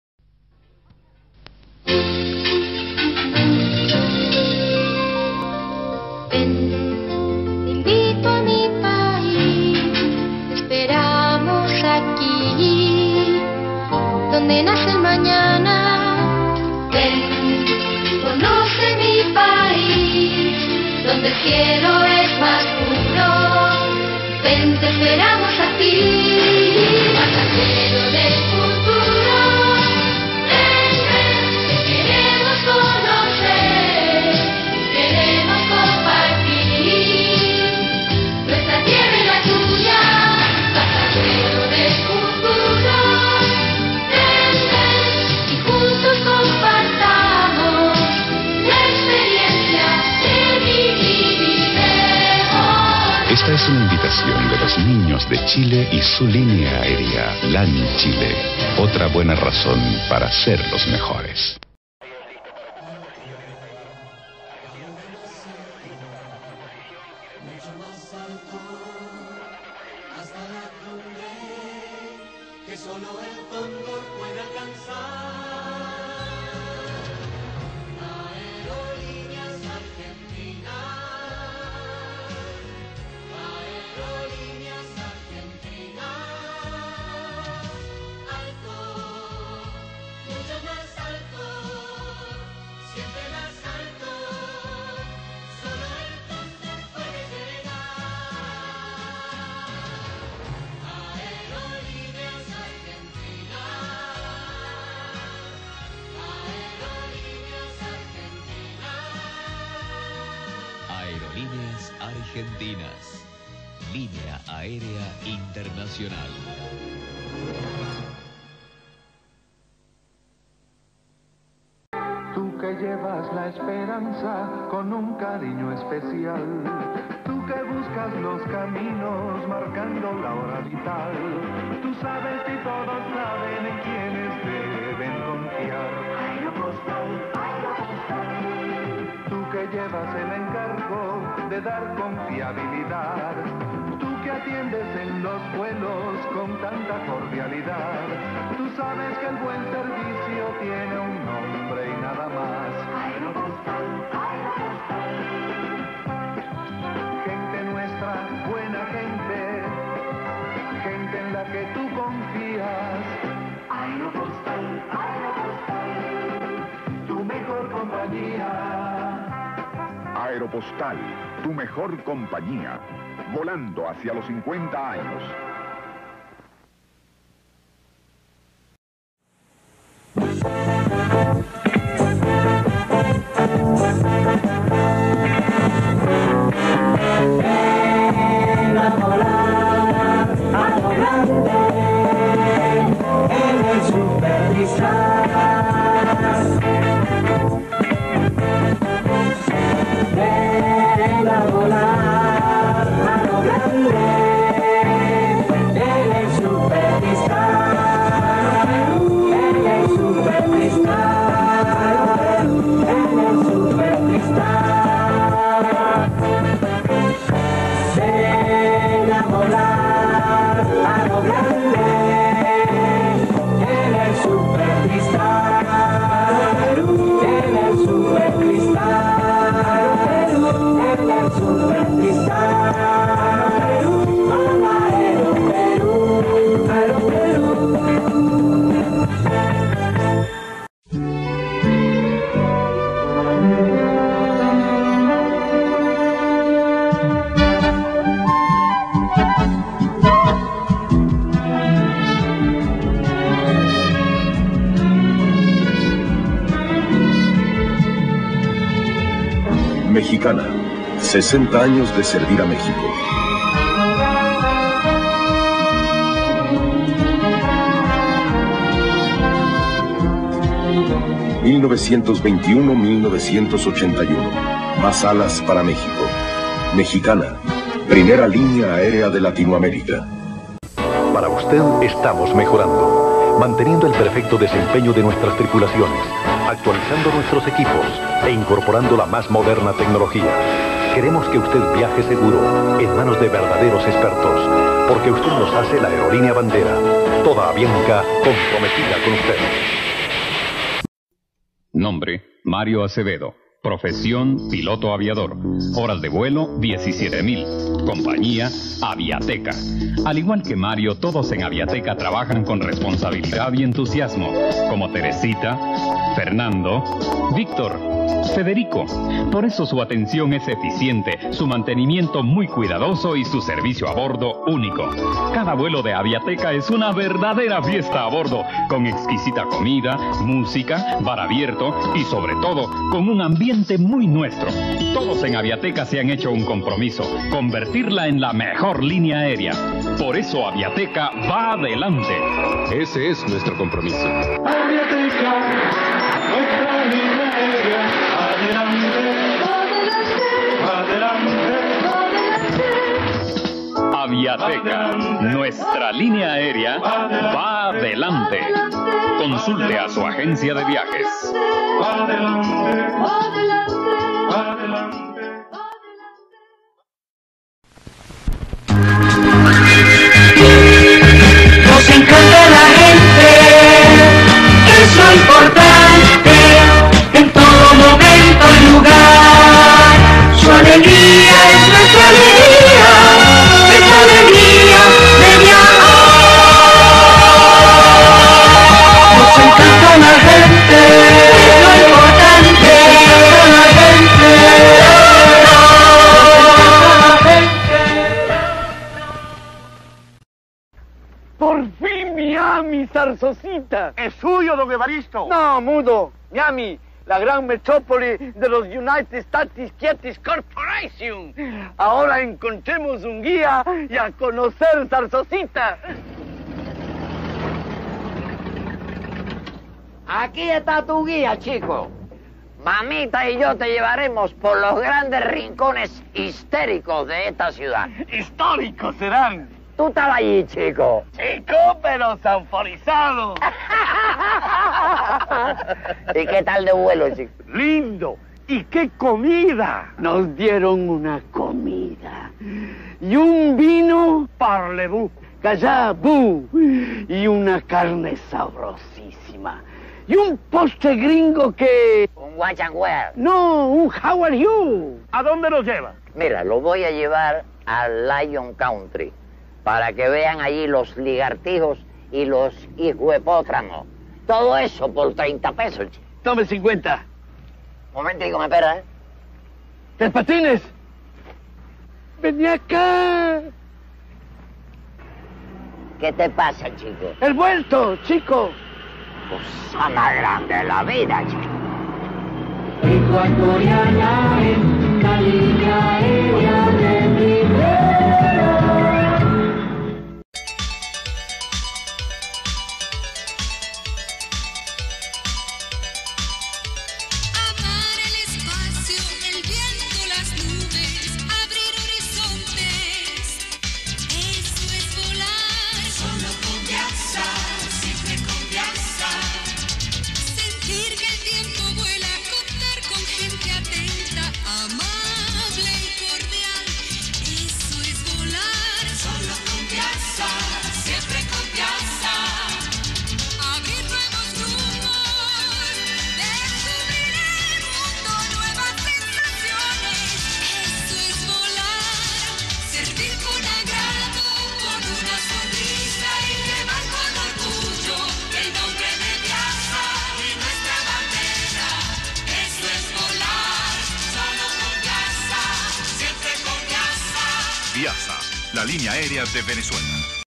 COMERCIAIS ANTIGOS | Companhias Aéreas da América Latina
Comerciais antigos de empresas aéreas latino-americanas:

1. LanChile
2. Aerolineas Argentinas
3. Aeropostal
4. Aeroperu
5. Mexicana
6. Avianca
7. Aviateca
8. Lacsa
9. Ecuatoriana de Aviacion
10.Viasa